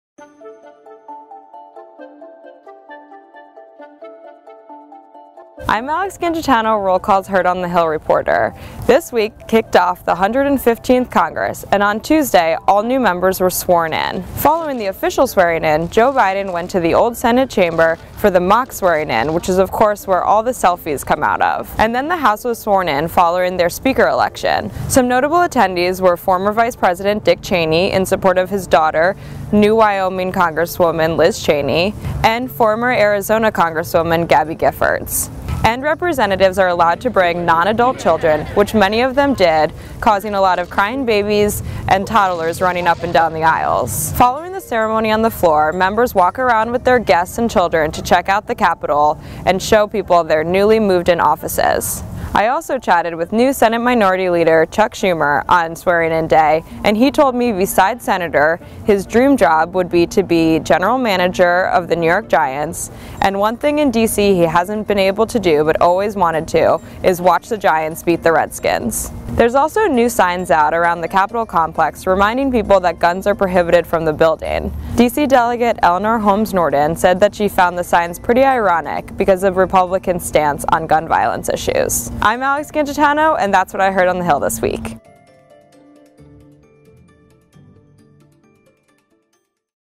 Heard on the Hill This Week: Swearing-In the 115th Congress
Out with the 114th and in the 115th Congress. Speaker Paul D. Ryan and Vice President Joseph R. Biden Jr. swore in the new members and Roll Call's Heard on the Hill reporter Alex Gangitano was all over the Capitol to fill you in on the proceedings.